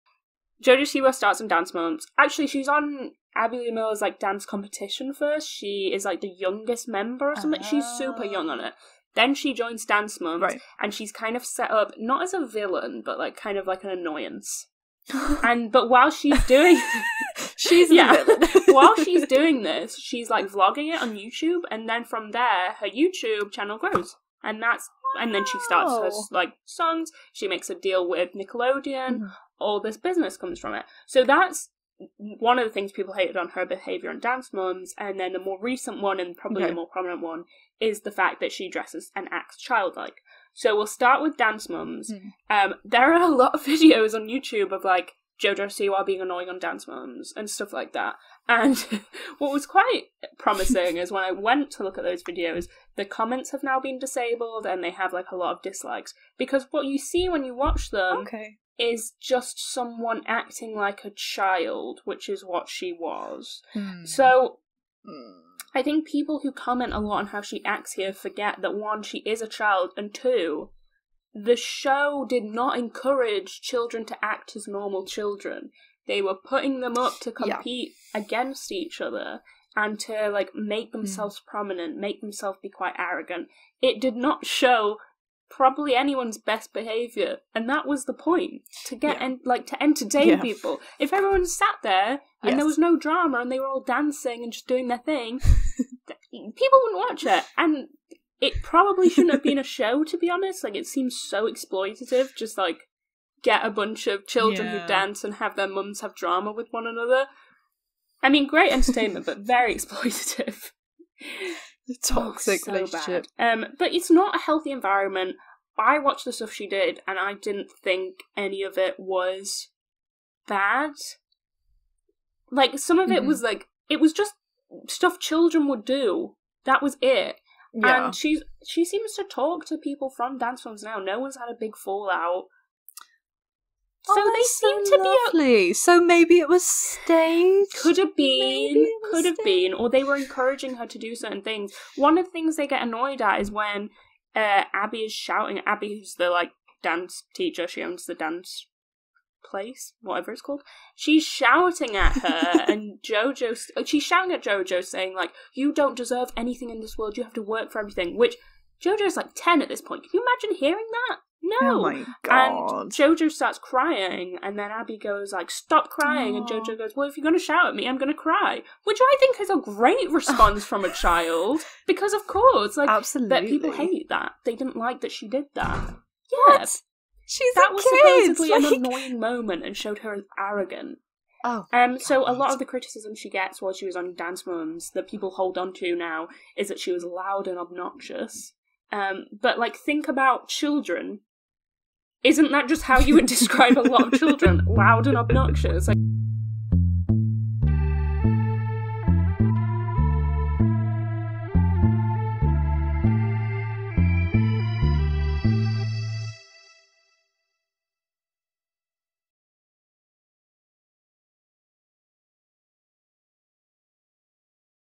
JoJo Siwa and Dance Moms
This week on Culture Hang we look at gen z child stars!
Hannah explains the public perception of JoJo Siwa which began with Dance Moms.
(Excuse the camera quality, we record through skype so it's not perfect lol)